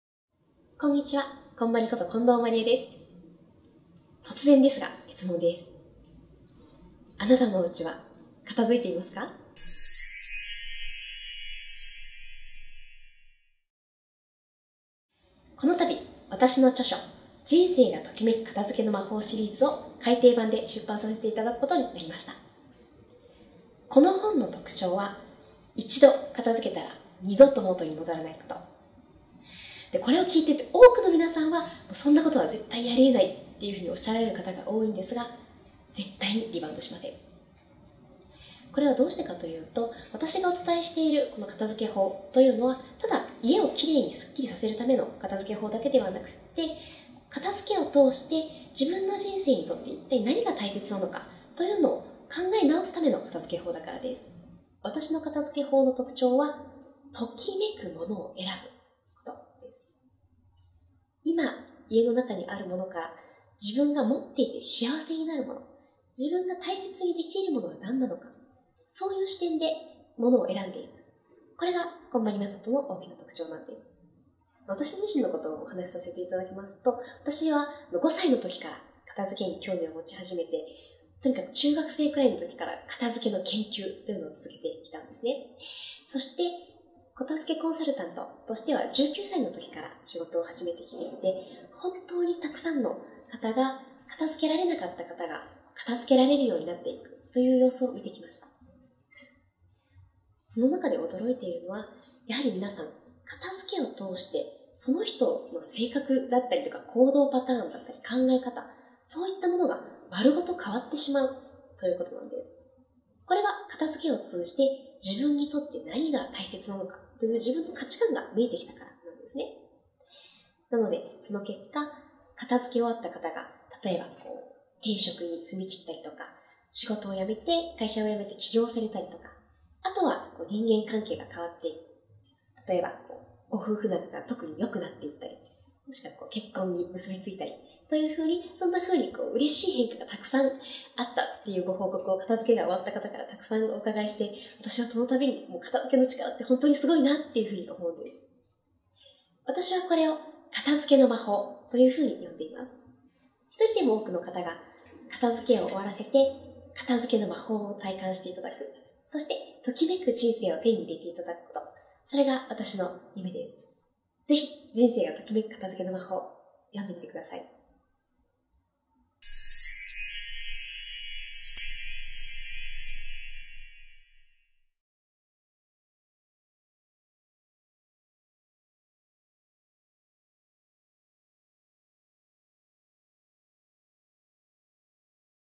近藤麻理恵『人生がときめく片づけの魔法』改訂版発売！
累計1000万部超えの世界的ベストセラー。
家の中を劇的に片づけると、人生も劇的に変わると話題のメソッド。
一度片づけたら絶対に元に戻らない、著者の原点となる本の最新版。